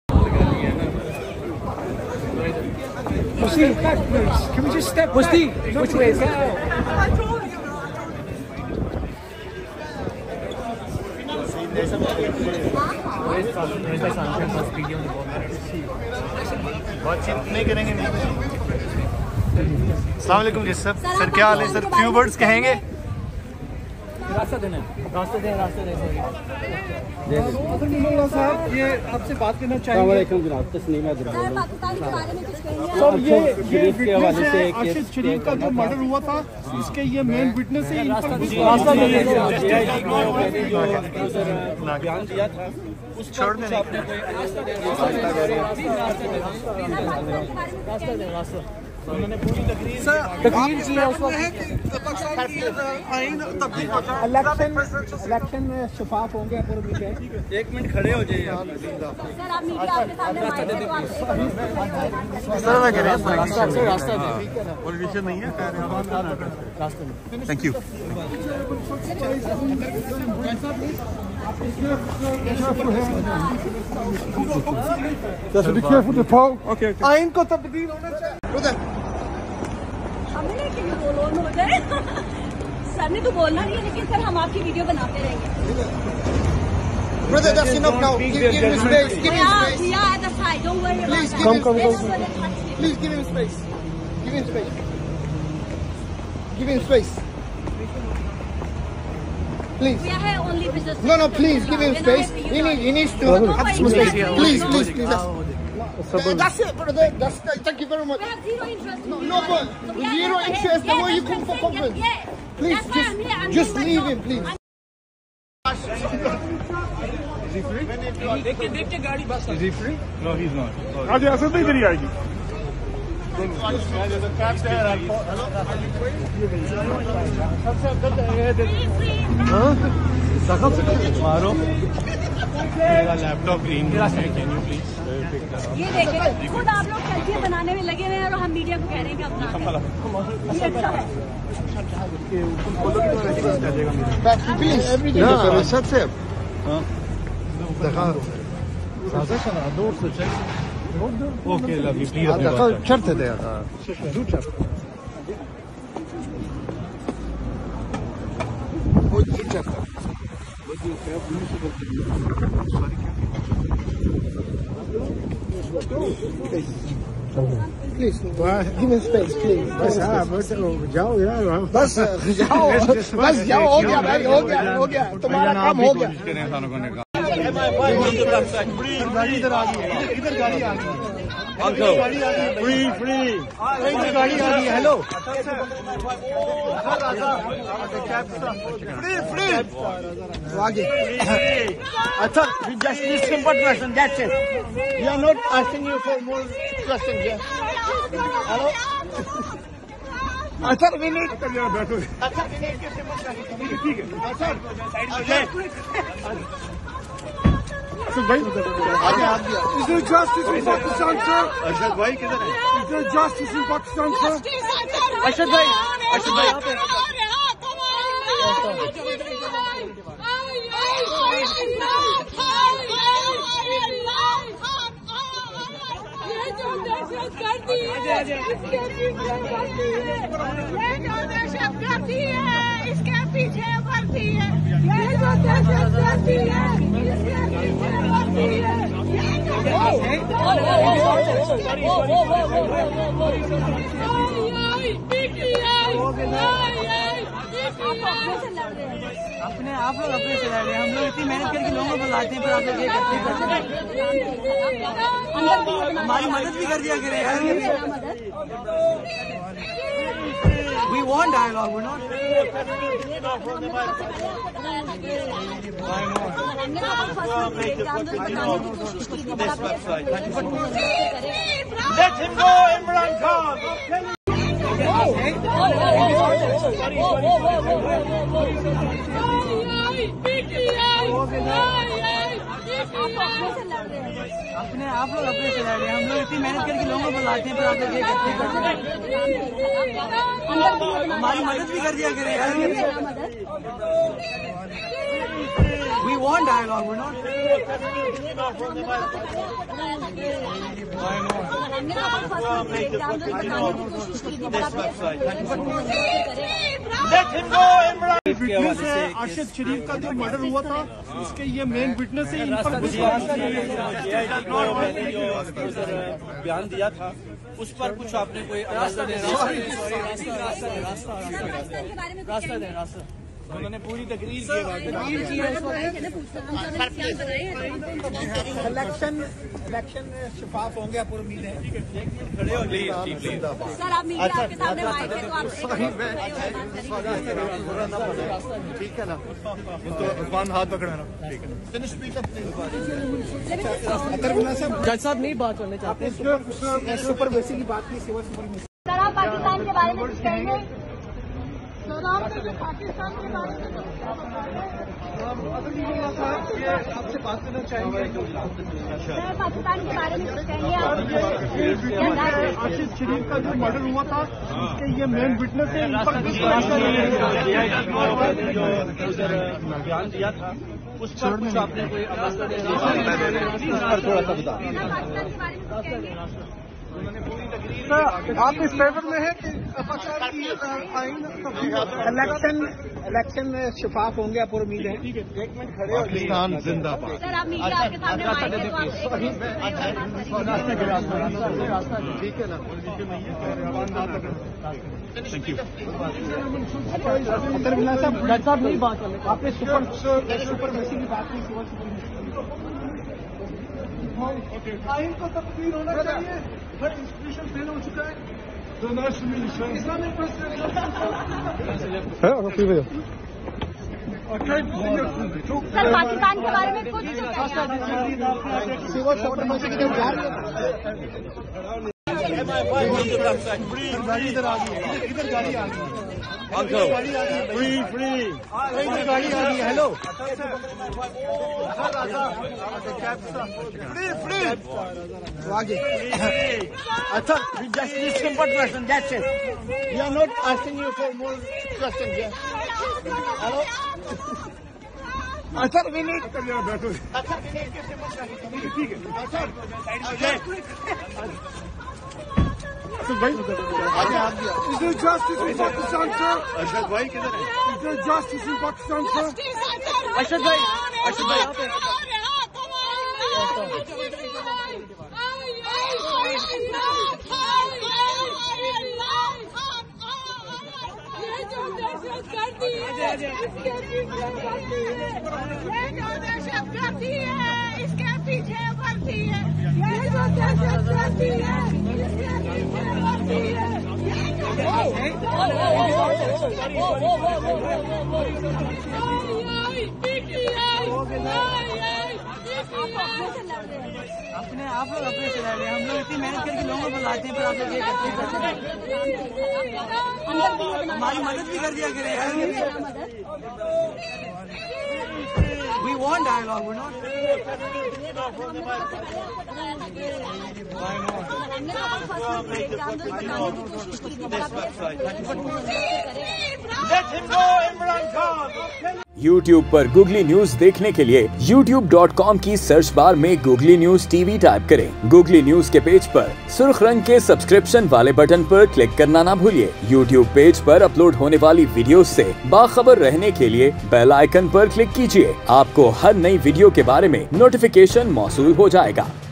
London Mein Imrandoos Kei Justice Athar Minallah Kai Sath Badtmeezi | Googly News TV
London Mein Imrandoos Kei Justice Athar Minallah Kai Sath Badtmeezi